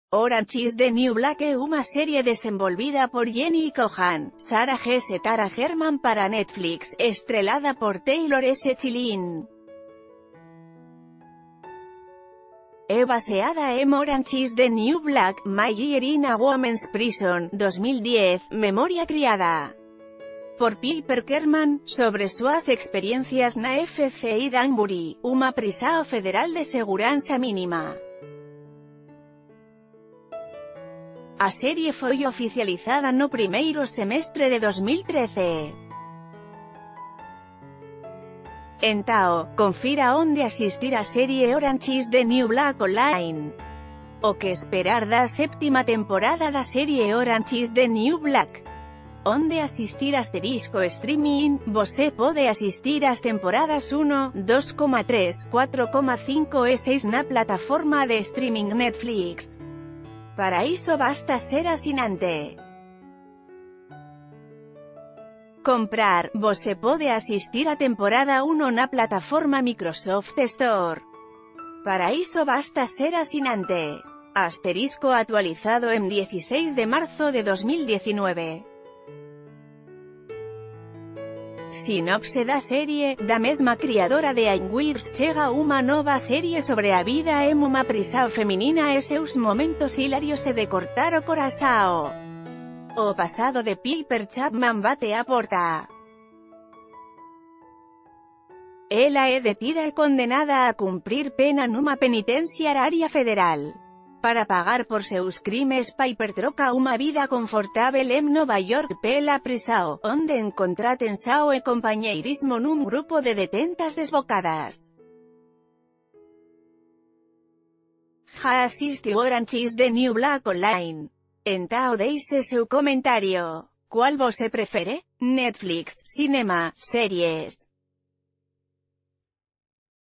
Onde Assistir | Série Orange Is the New Black online
Orange Is the New Black é uma série desenvolvida por Jenji Kohan, Sara Hess e Tara Herrmann para a Netflix, estrelada por Taylor Schilling. É baseada em Orange Is the New Black: My Year in a Women’s P...